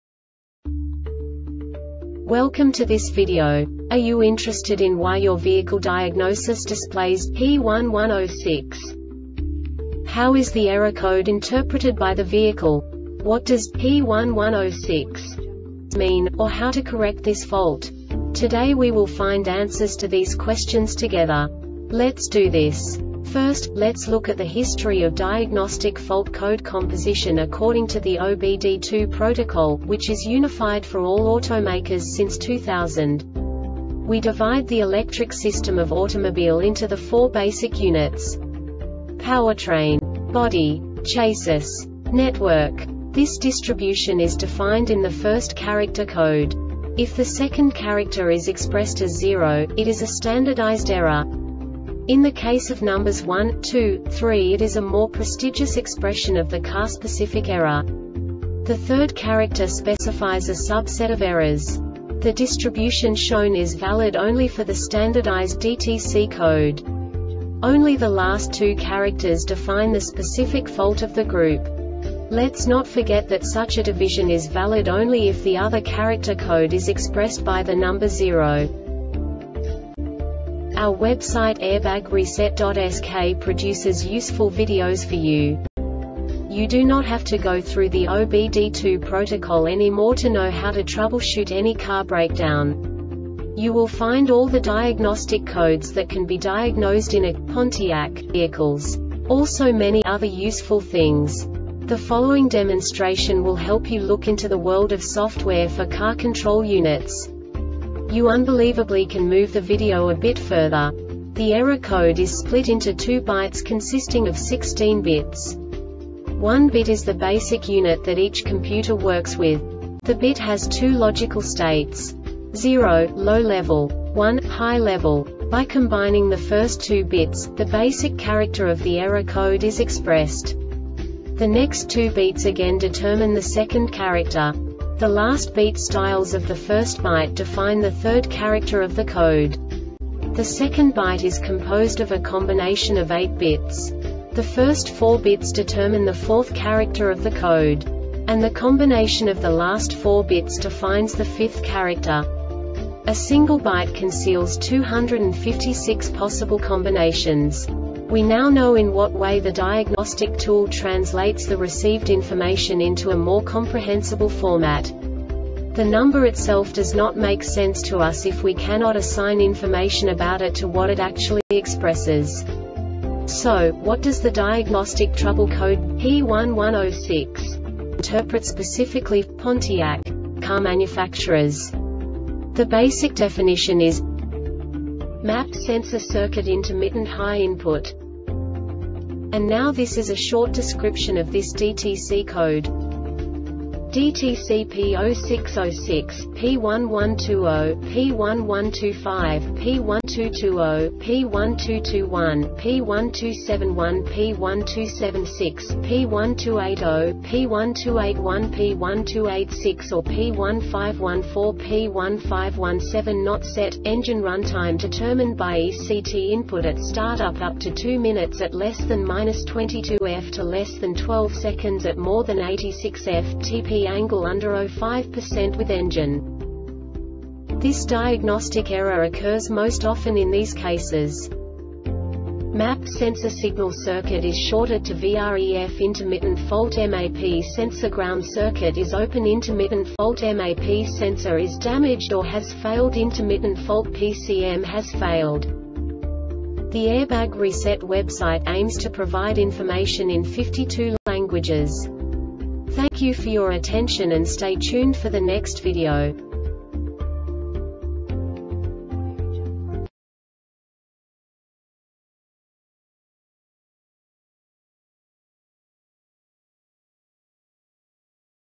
DTC Pontiac P1106 Short Explanation
Contents:
0:21 Basic DTC analysis according to OBD2 protocol standard.
1:48 Insight into programming
2:58 A diagnostically understandable description of the Diagnostic Trouble Code
3:05 Basic error definition

Make:
Pontiac
Code:
P1106
Definition:
MAP Sensor Circuit Intermittent High Input
Description:
DTC P0606, P1120, P1125, P1220, P1221, P1271-P1276, P1280, P1281-P1286 or P1514-P1517 not set,engine runtime determined by ECT input at startup (up to 2 minutes at less than -22ºF to less than 12 seconds at more than 86ºF),TP angle under 0.5% with engin
Cause:
MAP sensor signal circuit is shorted to VREF (intermittent fault)MAP sensor ground circuit is open (intermittent fault)MAP sensor is damaged or has failed (intermittent fault)PCM has failed